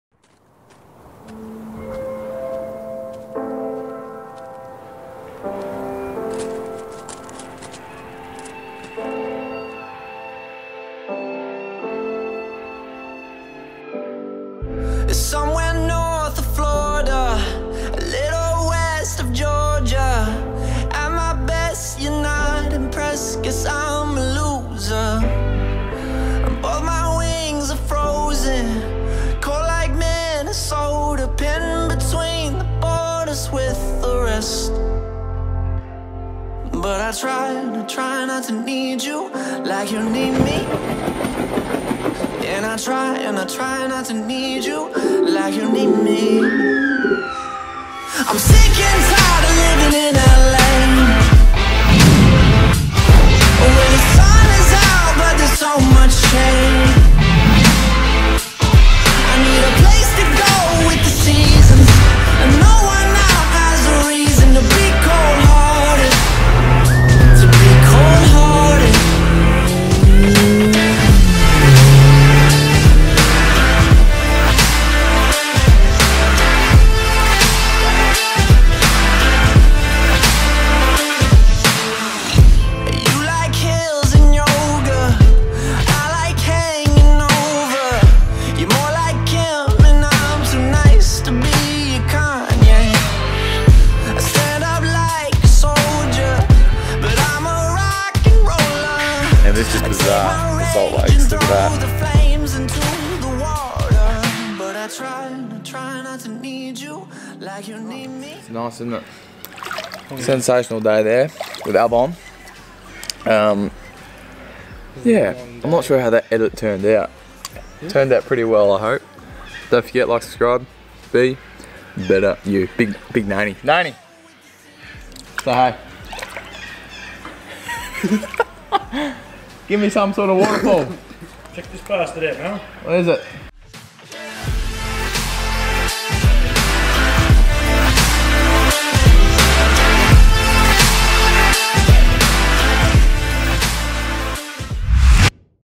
The Wimmera
The silo art trail is considered the largest outdoor gallery in Australia. It is a spectacular art display that serves as a reminder of the history and industry that has driven Australia for so many years.

Last weekend Alvin (@alvin_adpd) and I caught up and took a bit of a road trip through the Grampians and Wimmera region stopping to check out the silo art at Rupanyup and Sheep Hills, and the Pink Salt Lake at Dimboola. It was a spectacular sight and something that I definitely recommend doing if you get the chance!

I hope that you enjoy this video and a glimpse of the beautiful Wimmera Region.

- Josh Driscoll Gym Owner @ Iron Oak & Origin Athletics